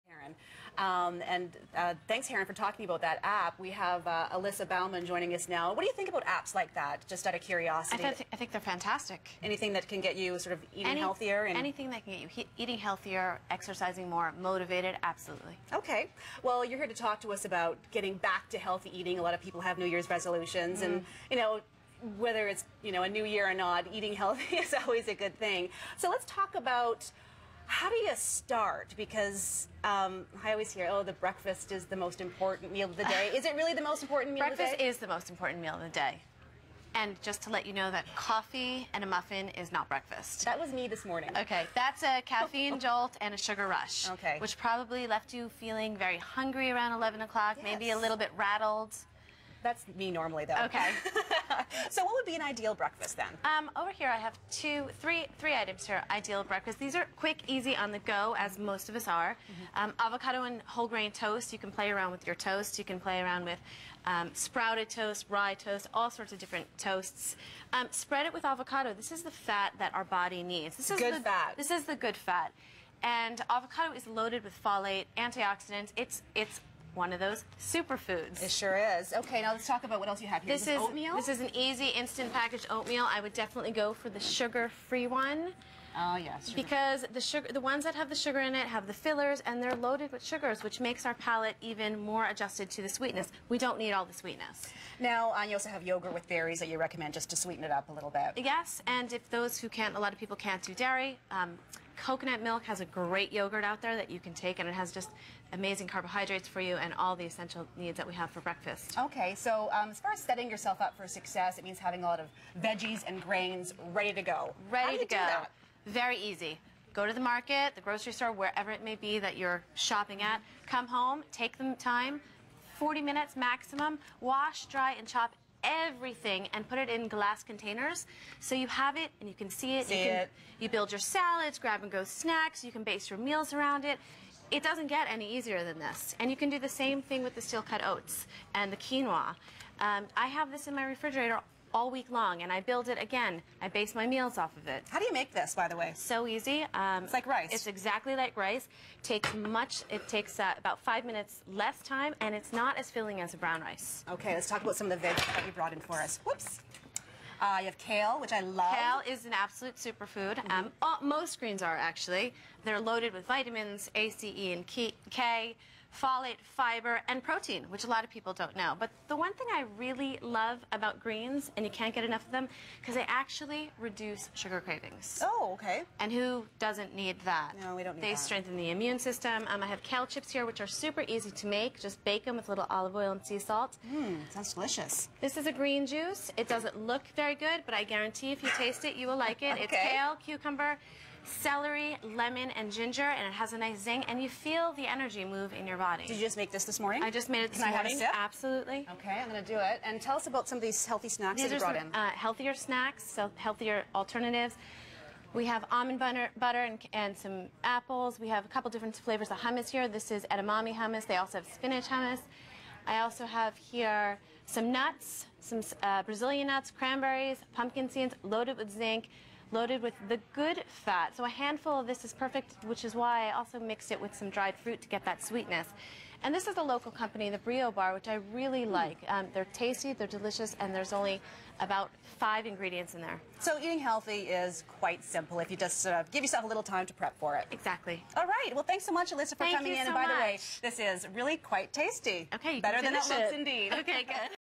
Incorporate super foods in your daily diet: nutritionist Alyssa Bauman
Incorporate super foods in your daily diet:  nutritionist Alyssa Bauman explains how